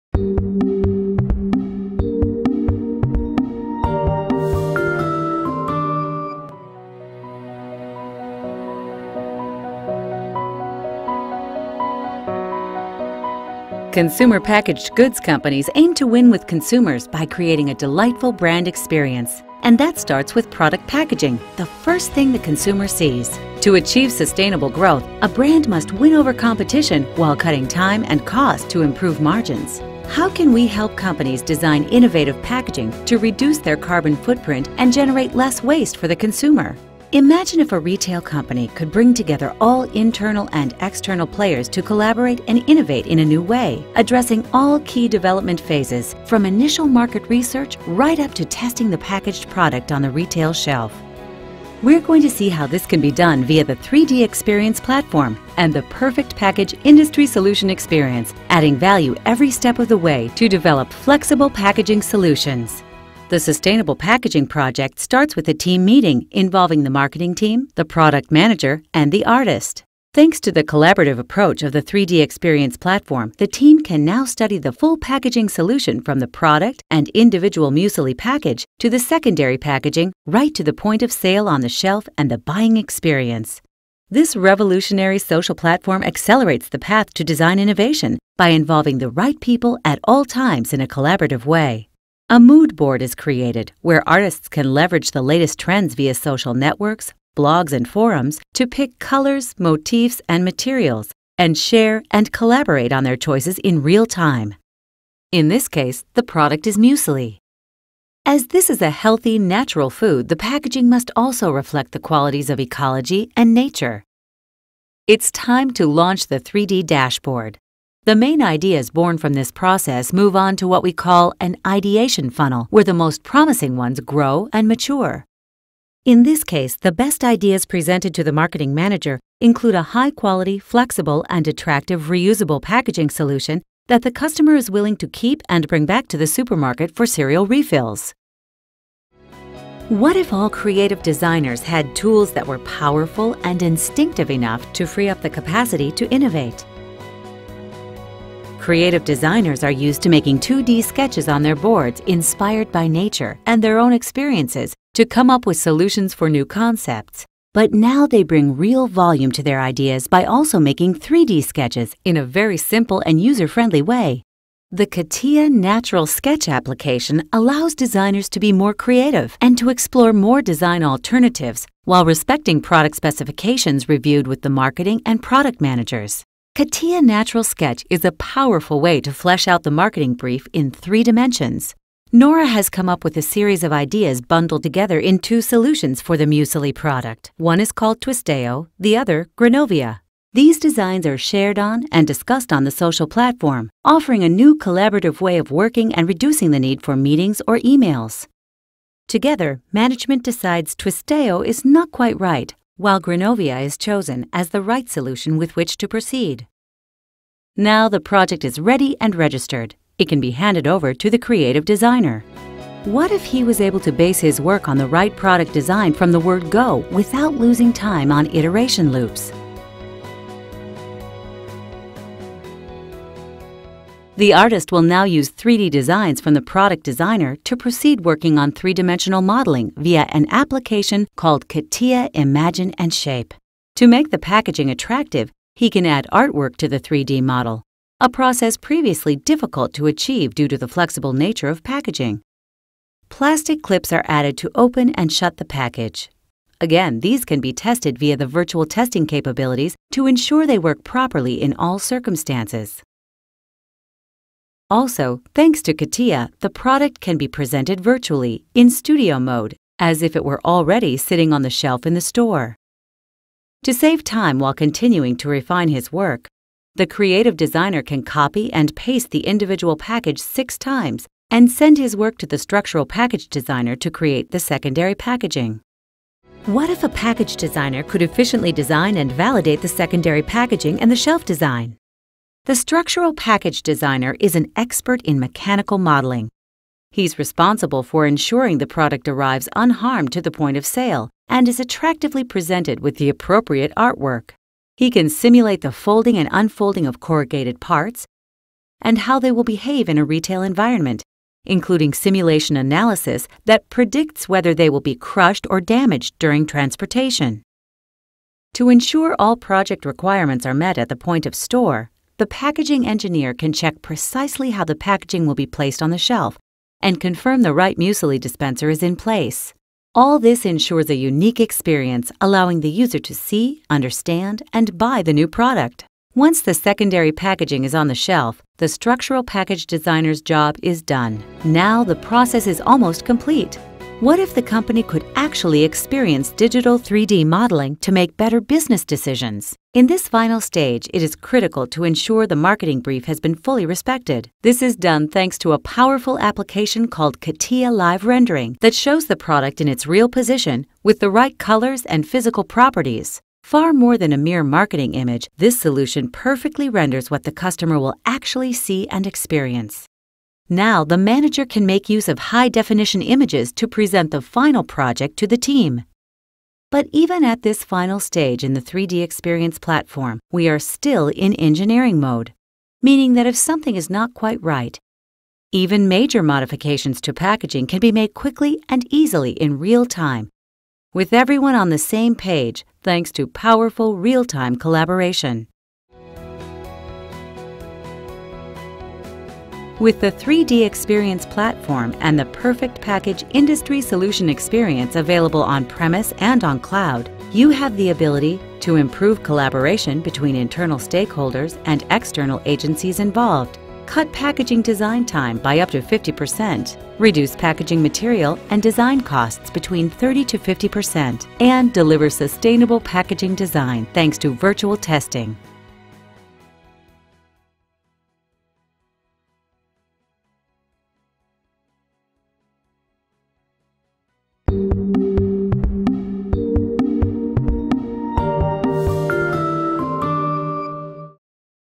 Perfect Packaging Concept and Design - Dassault Systèmes
Perfect Packaging Concept and Design - Dassault Systèmes. Discover how Perfect Package 3DEXPERIENCE from Dassault Systèmes provides the platform for packaging designers, engineers, and marketings to move quickly from concept to hyper-realistic 3D rendering to make faster decisions and reduce the time to market for new designs.